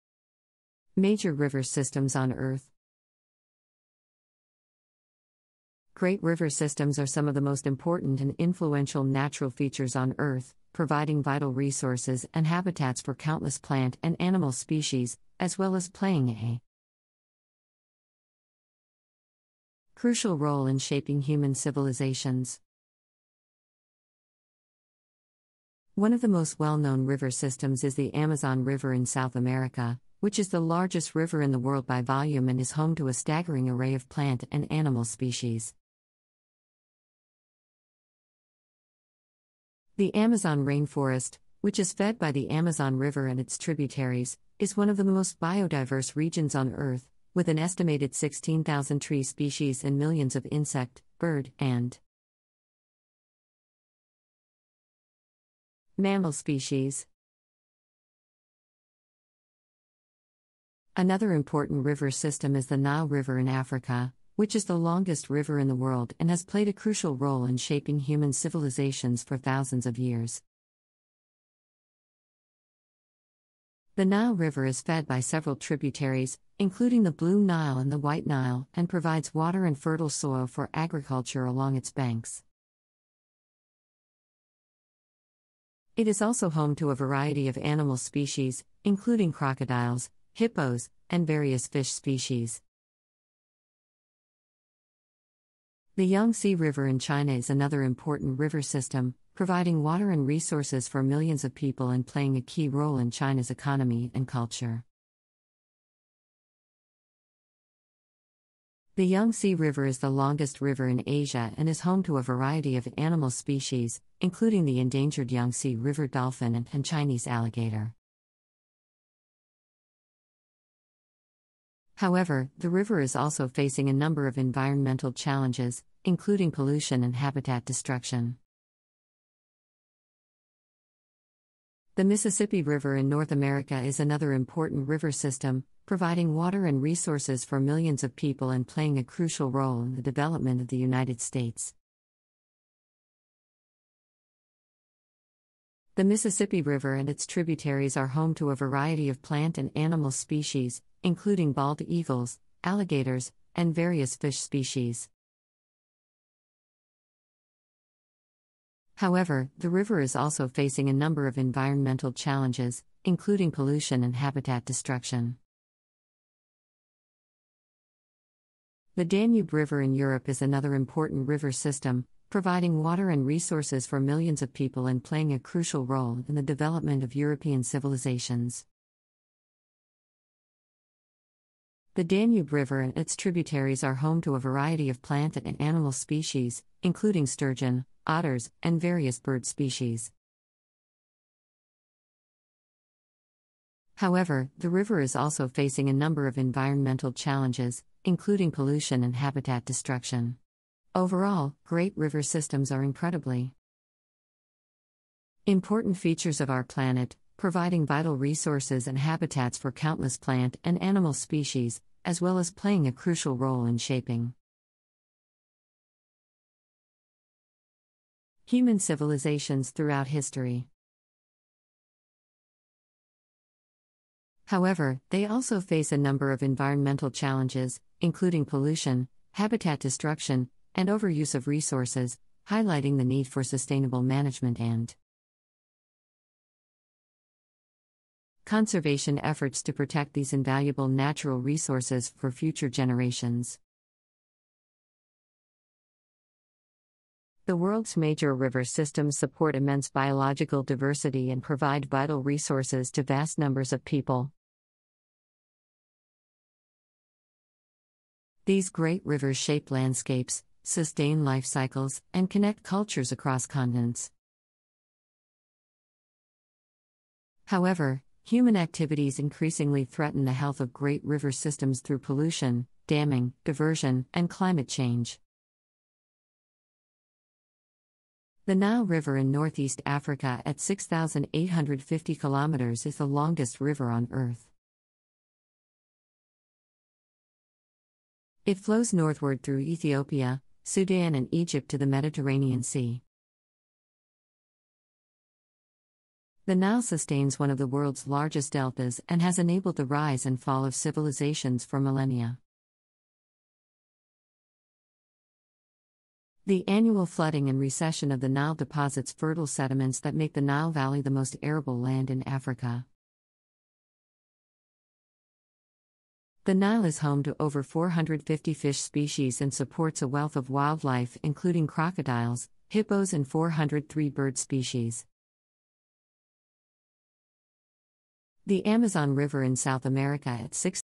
Major river systems on Earth | Part 1 | World Geography
Great river systems are some of the most important and influential natural features on Earth, providing vital resources and habitats for countless plant and animal species, as well as playing a crucial role in shaping human civilizations. One of the most well-known river systems is the Amazon River in South America, which is the largest river in the world by volume and is home to a staggering array of plant and animal species. The Amazon rainforest, which is fed by the Amazon River and its tributaries, is one of the most biodiverse regions on Earth, with an estimated 16,000 tree species and millions of insect, bird, and mammal species.